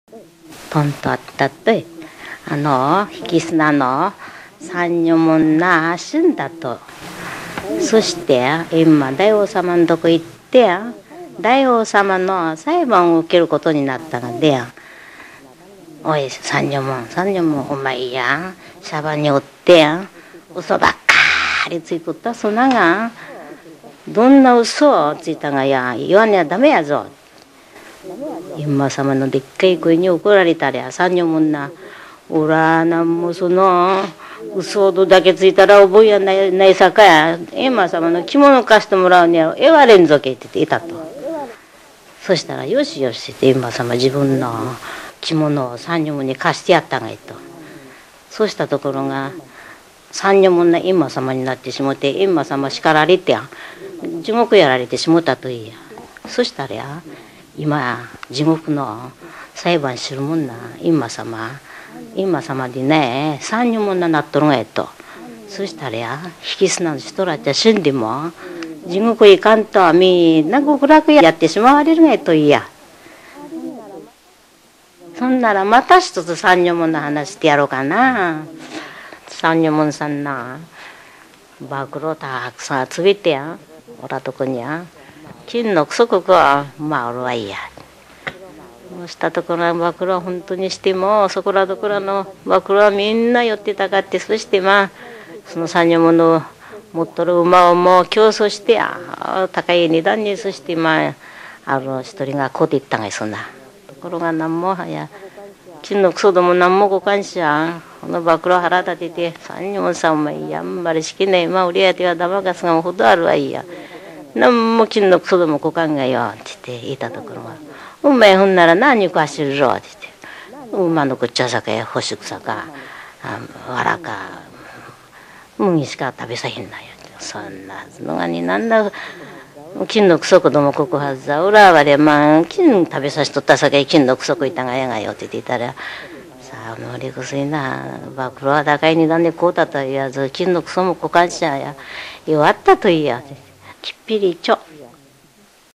能登弁で聞く民話「さんにょもん」
誰でも知ってる三右衛門さんの とんち話、「閻魔様で困らせて、なんとま～」　石川県珠洲市三砂町引砂